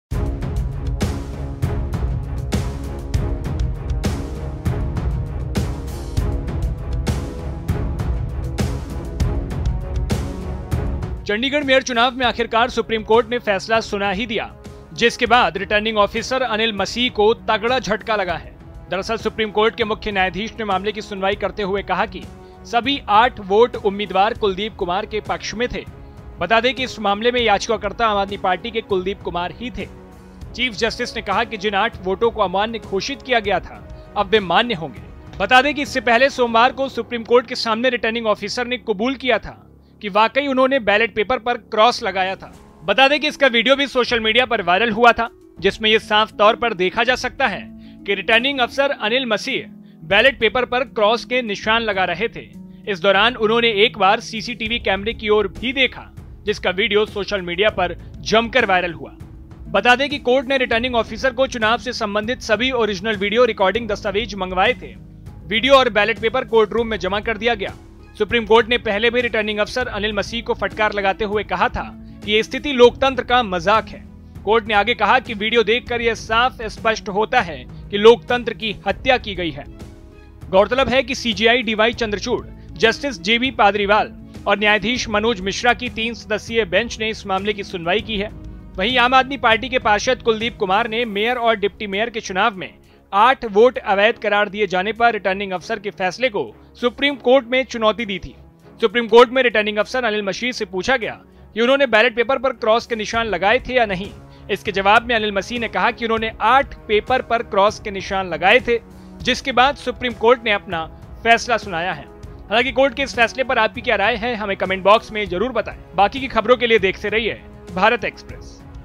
Chandigarh Mayor Election में SC ने सुनाया फैसला | INDIA Alliance | BJP | AAP
चंडीगढ़ मेयर चुनाव मामले में आखिरकार सुप्रीमकोर्ट ने फैसला सुना ही दिया... जिसके बाद बीजेपी को तगड़ा झटका लगा है...  दरअसल, सुप्रीम कोर्ट के मुख्य न्यायाधीश ने मामले की सुनवाई करते हुए कहा कि सभी 8 वोट उम्मीदवार कुलदीप कुमार के पक्ष में थे. बता दें कि इस मामले में याचिकाकर्ता आम आदमी पार्टी के कुलदीप कुमार ही थे.

Producer -  Ujjwal
Editor- Ishika